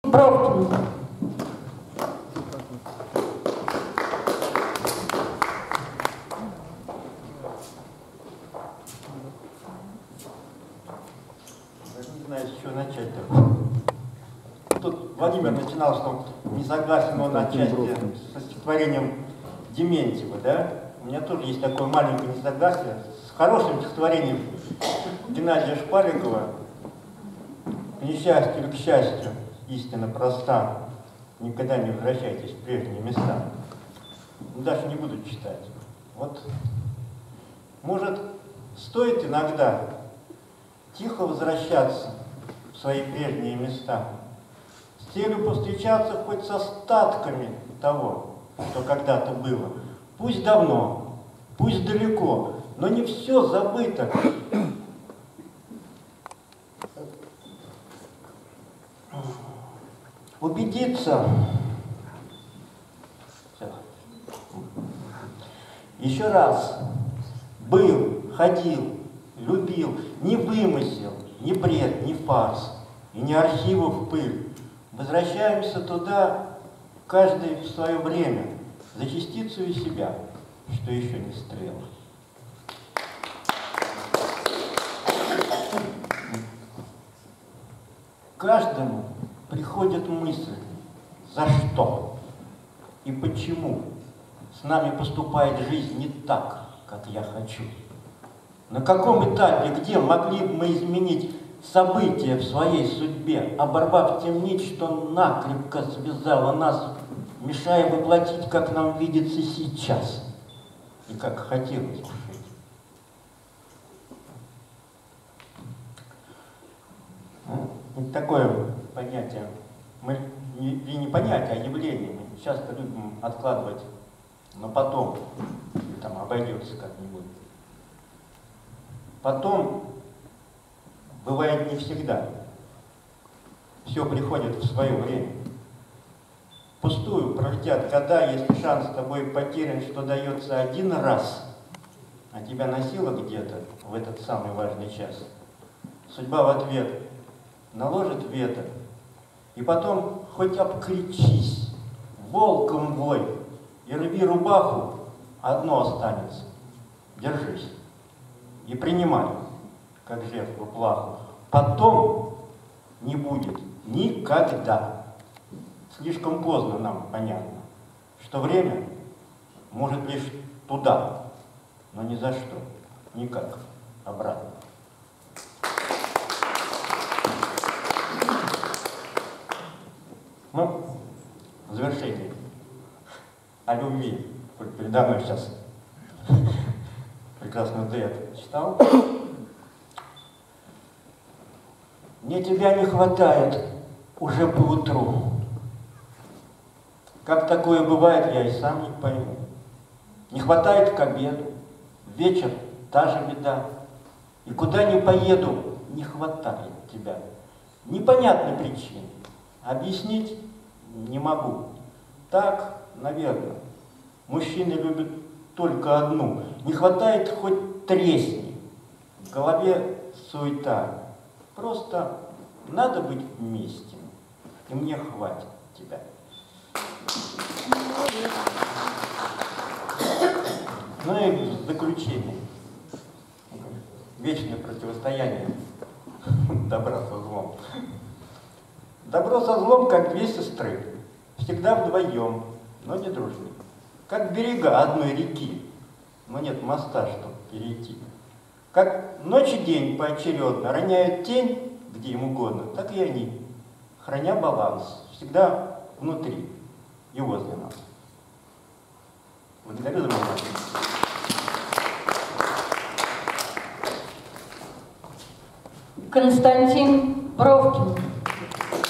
Константин Бровкин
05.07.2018 И снова с нами салон Творчества "Неравнодушие" Елены Жмачинской, который прошёл в Центральном ботаническом саде РАН, Лабораторный корпус, конференц-зал